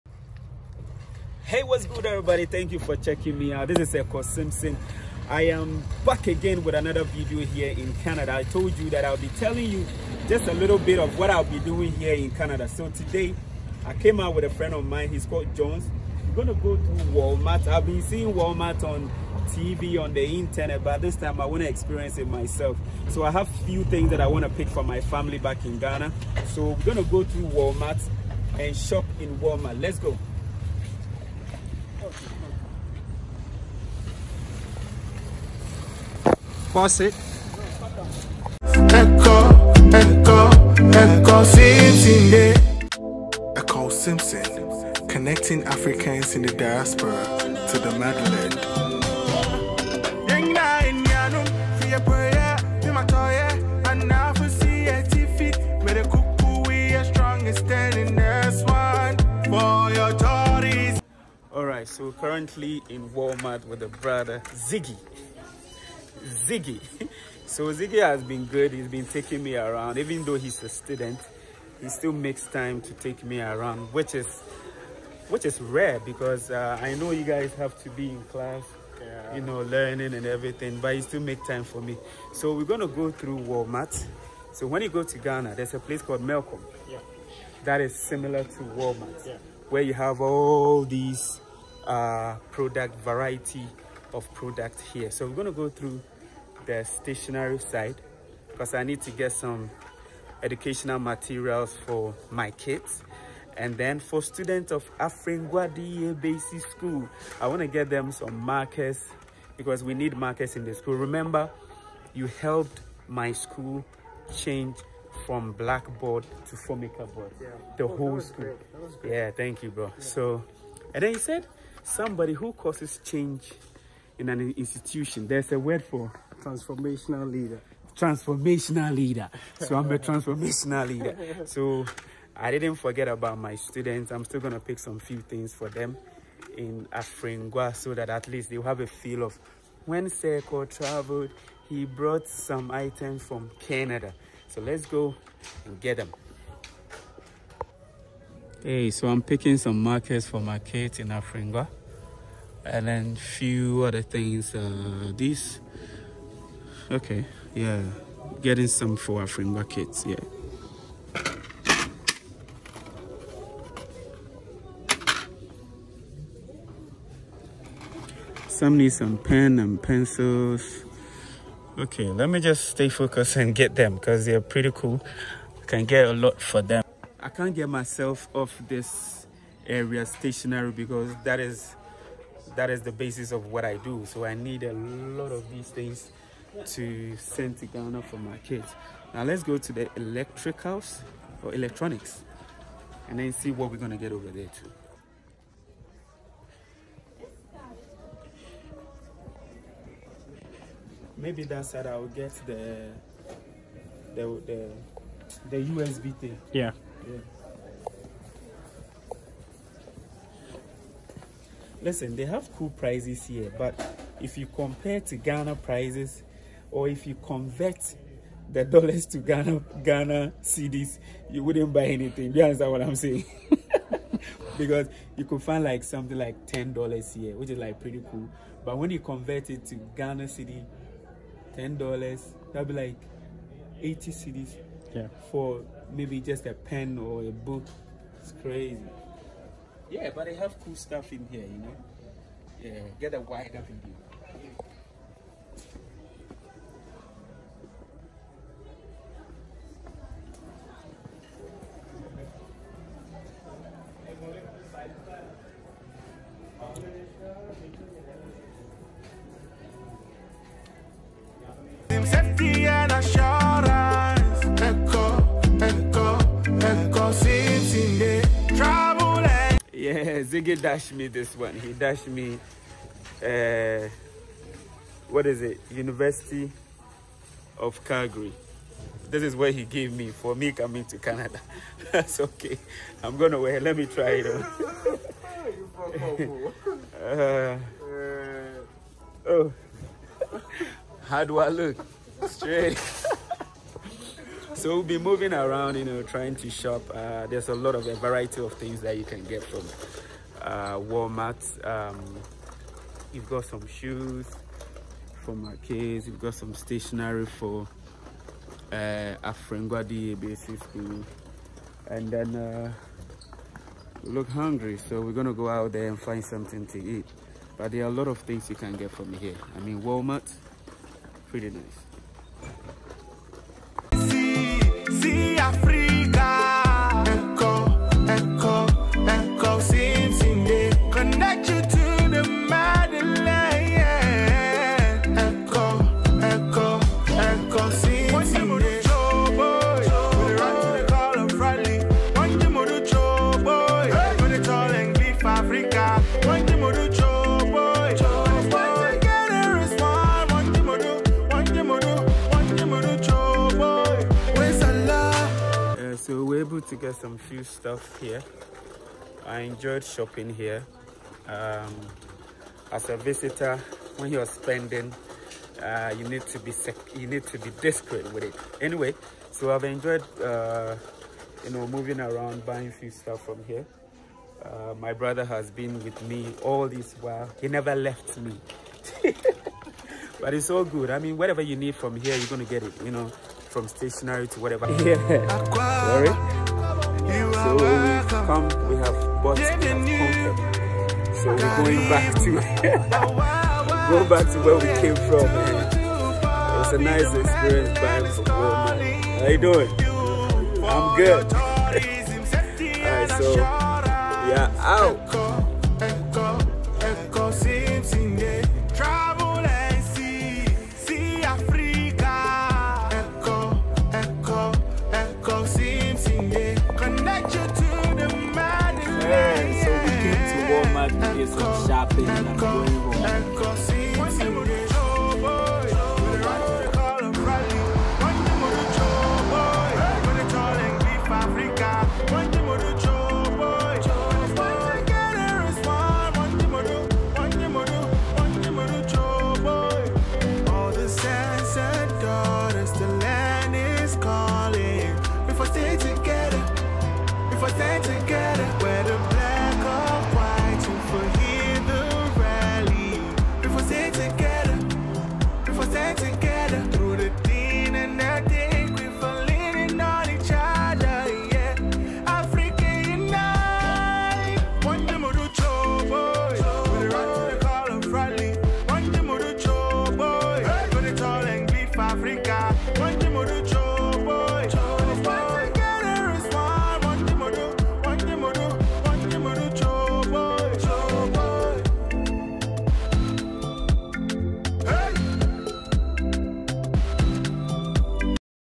WALMART Shopping In Calgary, Alberta, Canada🇨🇦 2023 |Canada vlog #4
WALMART Shopping Tour In Calgary in Alberta Canada 🇨🇦 2023 canadavlogs .

Shopping And Touring WALMART Supermarket In Calgary, Canada 🇨🇦. It was an awesome experience to have visited and shopped at Canada's supermarket Walmart in Calgary.